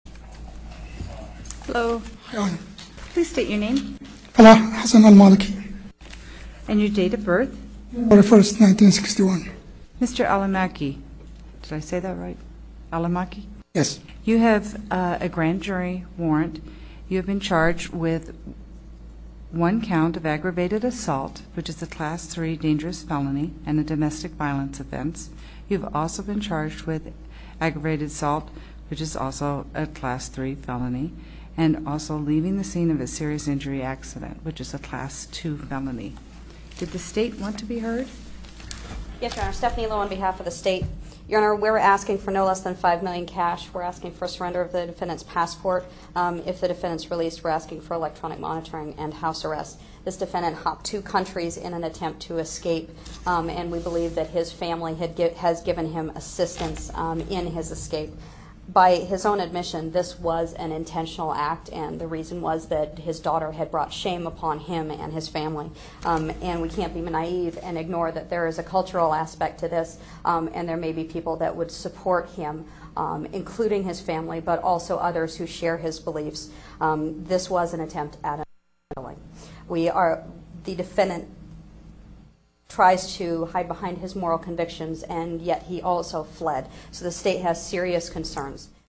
Raw Video: Ariz. 'honor-killing' Dad in Court
A Glendale father sits in jail as his family mourns the loss of his 20-year-old daughter killed in a hit-and-run crash. Family members say Faleh Hassan Almaleki believed his daughter was not living according to traditional Iraqi values. (Nov. 3)